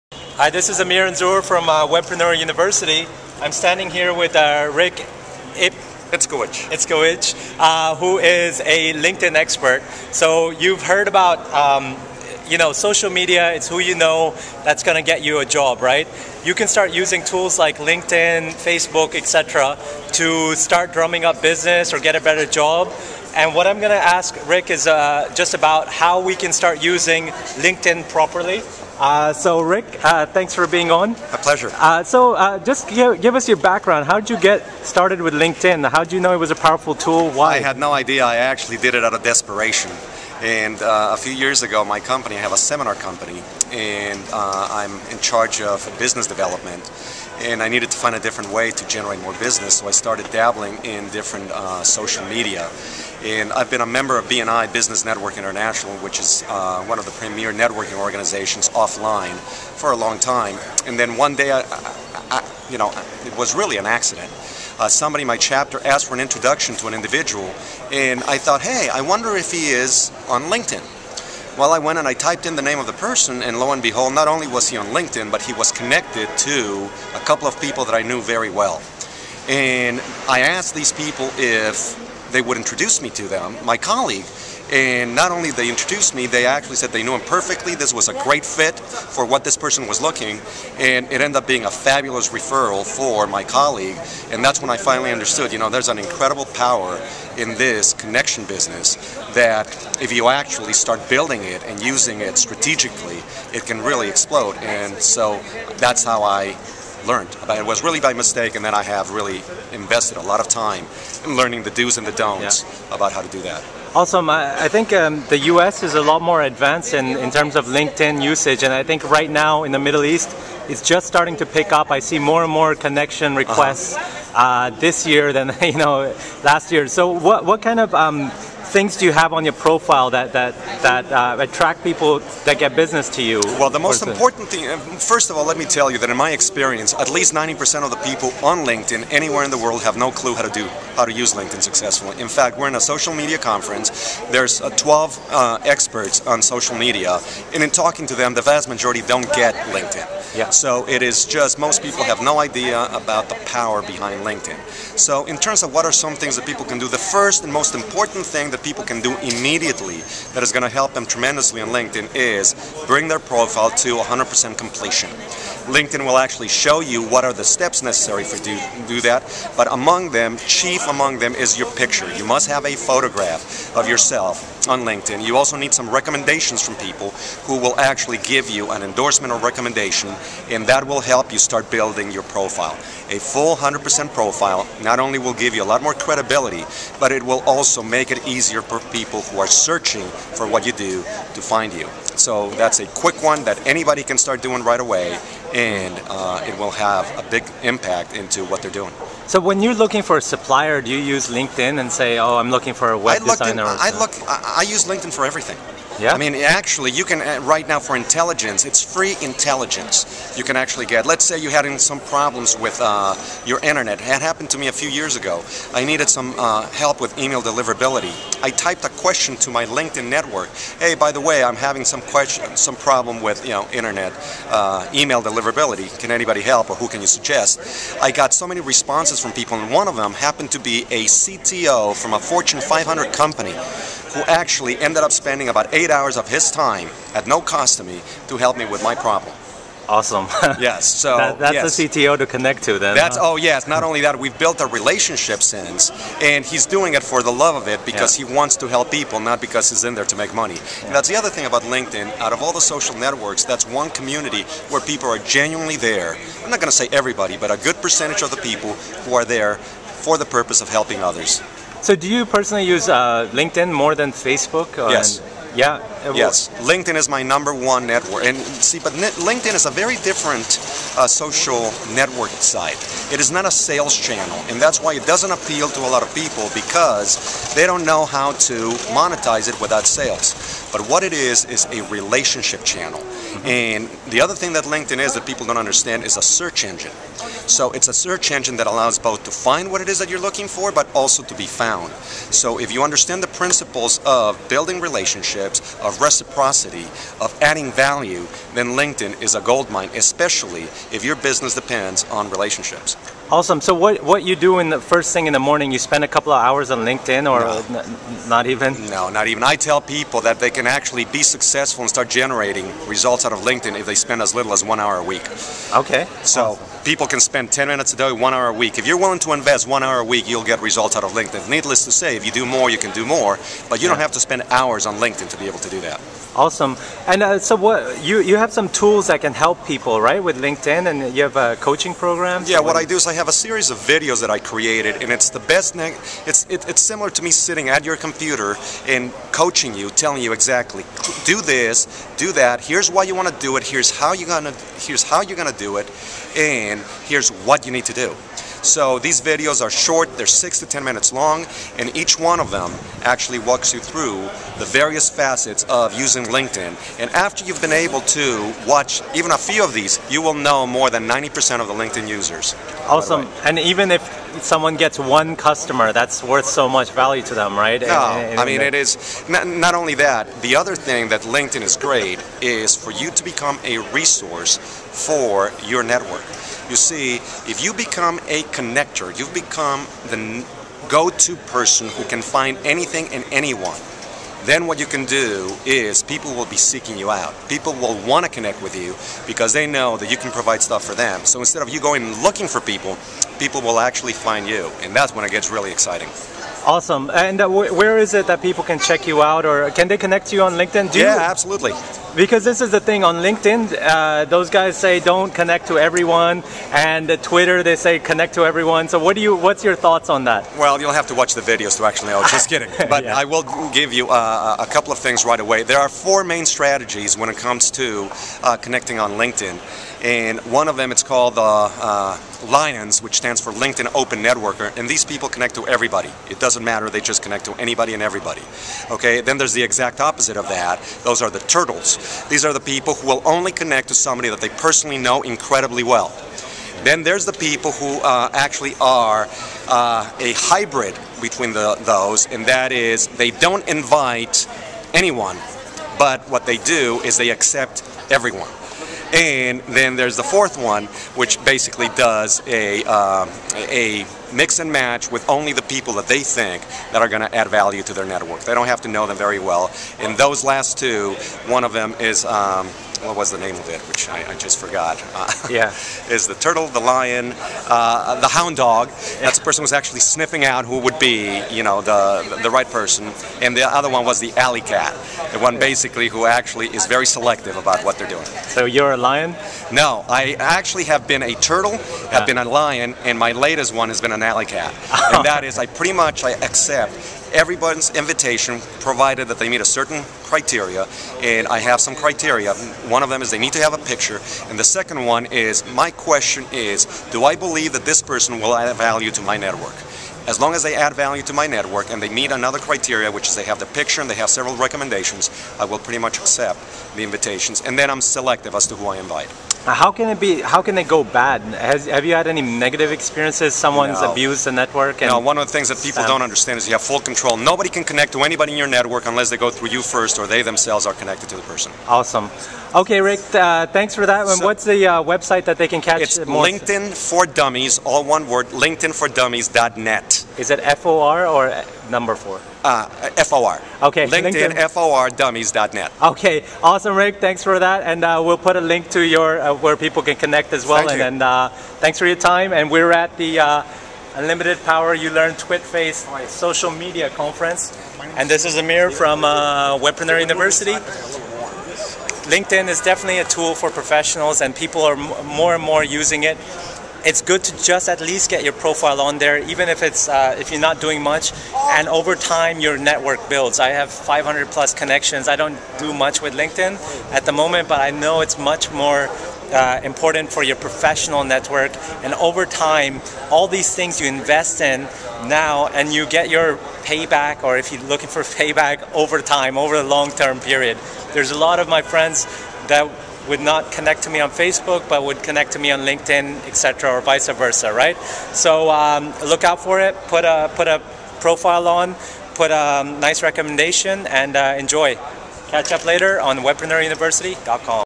How to use LinkedIn Effectively
Rick Itzkowich is a LinkedIn expert and teaches people how to use LinkedIn effectively.  LinkedIn is a great relationship channel as well as a search engine for people.

Rick talks about the Lions, turtles, hound dog and alley cat strategy.